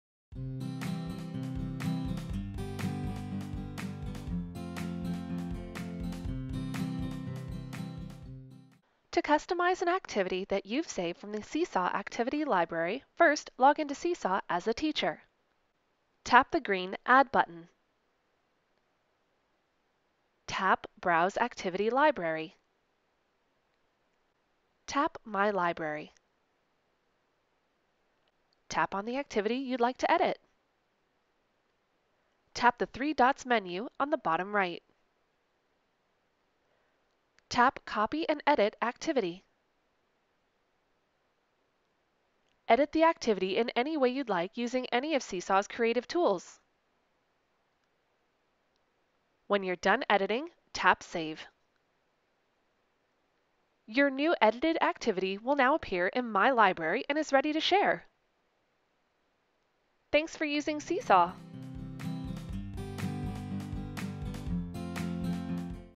Edit and customize activities from the Activity Library in Seesaw
This video will show you how to edit activities in Seesaw!

To learn more about Seesaw and Seesaw for Schools, visit web.seesaw.me